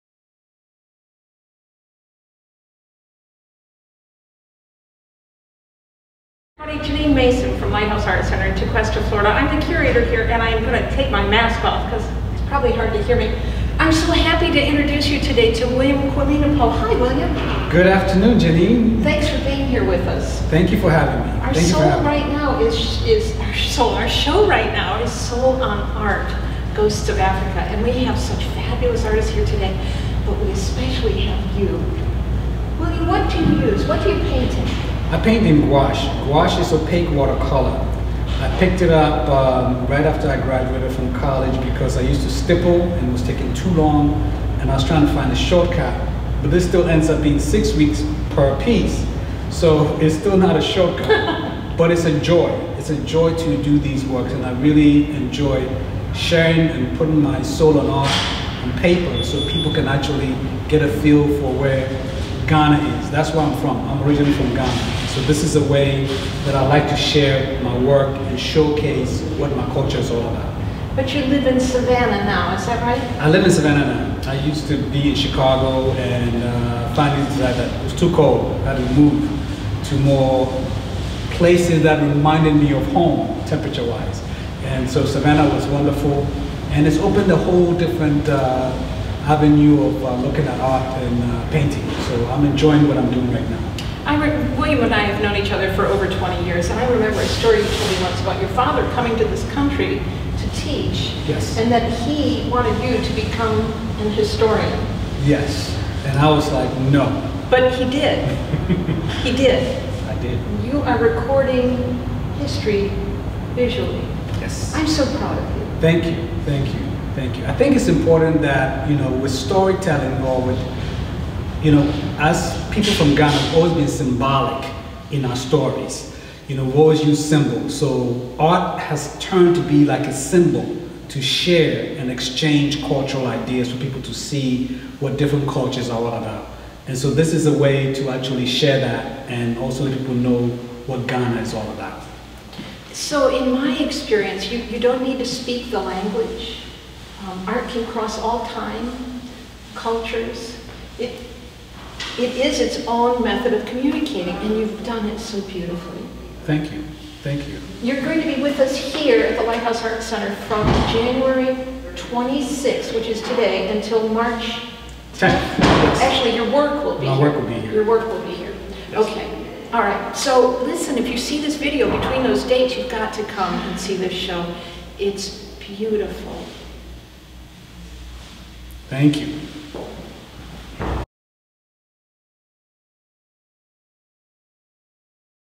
William Kwamena-Poh & Janeen Mason discuss Soul on Art - Lighthouse ArtCenter
Lighthouse ArtCenter opens the 2021 season with Soul on Art: Ghosts of Africa, from January 26 – March 10.
The Lighthouse ArtCenter is honored to display evocative artworks showcasing the personal experience and cultural heritage of esteemed artists, whose work is held in institutions like the Metropolitan Museum of Art, the Solomon R. Guggenheim Museum, and others. The list on exhibition reads like the who's who of painters, sculptors, and multi-media black and brown artists in the world today, including Nick Cave, Leonardo Drew, Ellen Gallagher, Adam Pendleton, William Kwamena-Poh, Ransome, Kara Walker, and Purvis Young. While some of the pieces are on loan from private collections, many are available for purchase.